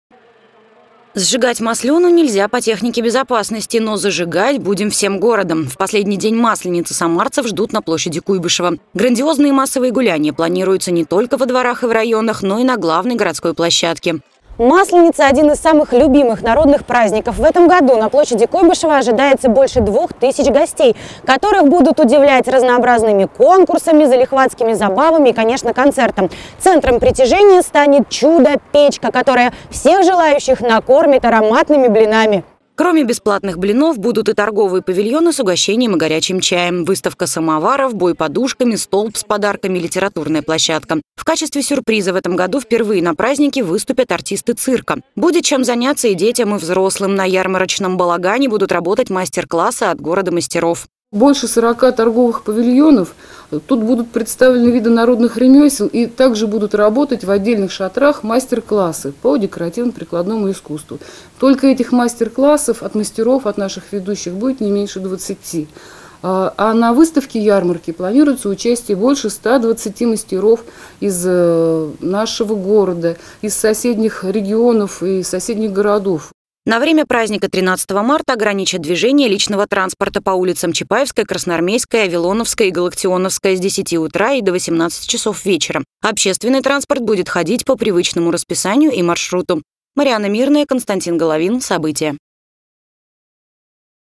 На масленичной неделе по всей Самаре пройдут праздничные гуляния
Несмотря на то, что Масленица – наследие языческой культуры, церковь определила для праздника своё название – Сырная седмица. Православные в эти дни едят все, кроме мяса. В Самаре жители ежегодно устраивают массовые гуляния. На неделе состоятся 68 дворовых и 22 районных праздника